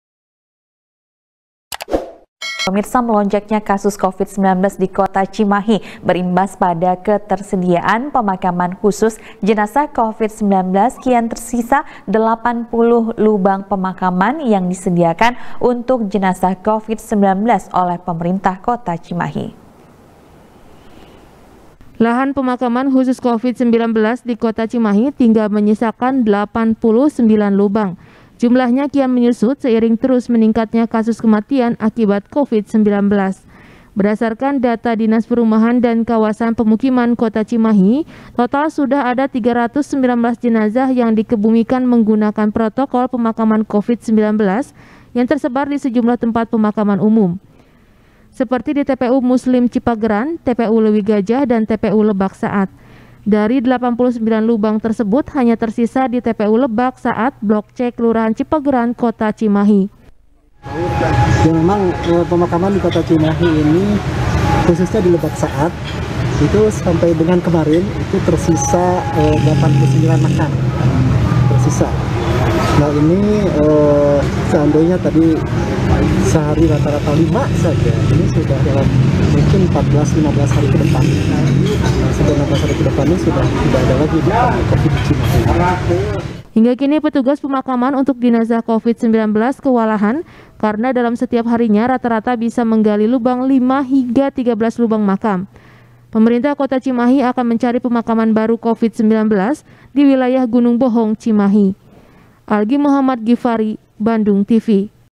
Lahan Pemakaman Covid-19 Di Cimahi Kian Menyusut | SBR | BANDUNGTV
SBR | BANDUNGTV | KRISANTI | melonjaknya kasus covid 19 di kota cimahi, berimbas pada ketersediaan pemakaman khusus jenazah Covid 19. Kini tersisa delapan puluh lubang pemakaman, yang disediakan untuk jenazah Covid-19 oleh Pemerintah Kota Cimahi.